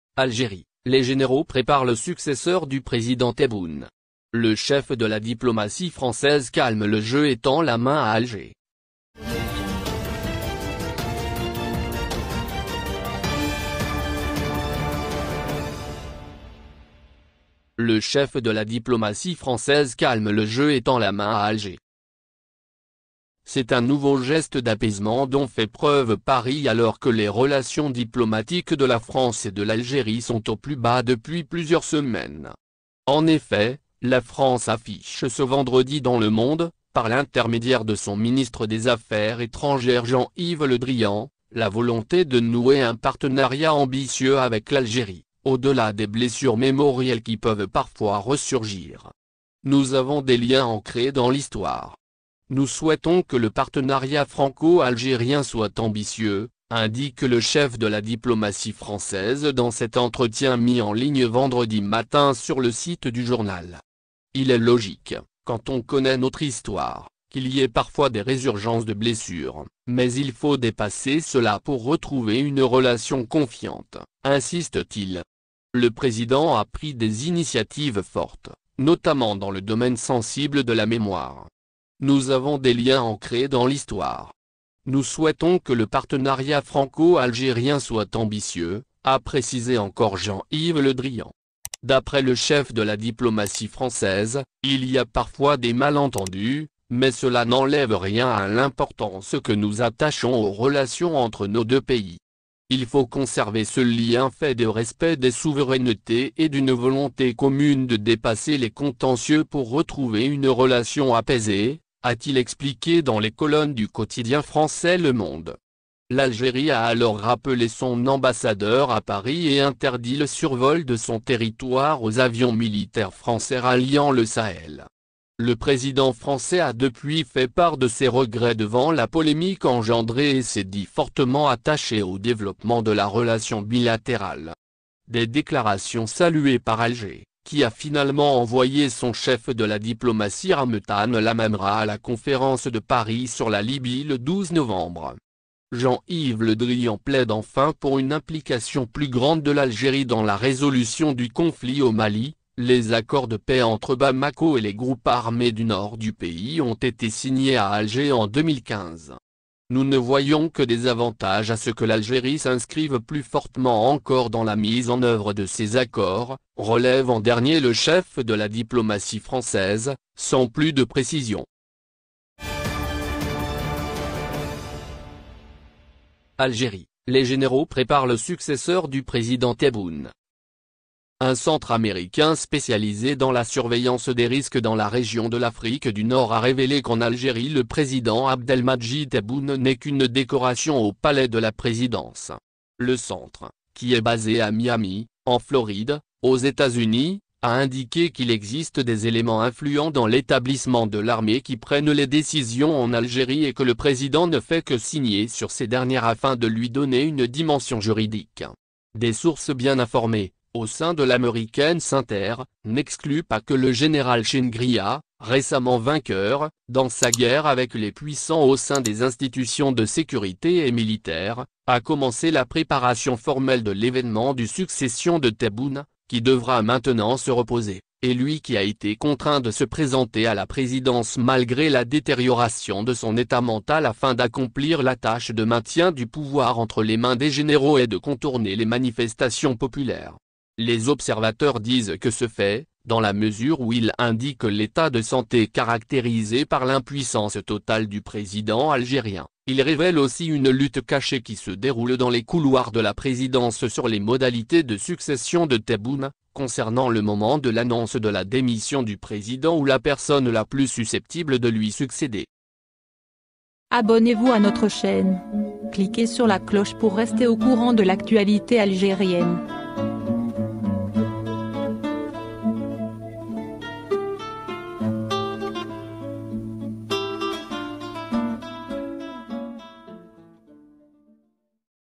Les généraux préparent le successeur de Tebboune | Le Drian calme le jeu et tend la main à Alger...
C’est un nouveau geste d’apaisement dont fait preuve Paris alors que les relations diplomatiques de la France et de l’Algérie sont au plus bas depuis plusieurs semaines. En effet, la France affiche ce vendredi dans Le Monde, par l’intermédiaire de son ministre des Affaires étrangères Jean-Yves Le Drian, la volonté de nouer un « partenariat ambitieux » avec l’Algérie, au-delà des « blessures » mémorielles qui peuvent « parfois » resurgir.
Un centre américain spécialisé dans la surveillance des risques dans la région de l’Afrique du Nord a révélé qu’en Algérie le président Abdelmadjid Tebboune n’est qu’une décoration au palais de la présidence.

Le centre, qui est basé à Miami, en Floride, aux États-Unis, a indiqué qu’il existe des éléments influents dans l’établissement de l’armée qui prennent les décisions en Algérie et que le président ne fait que signer sur ses dernières afin de lui donner une dimension juridique.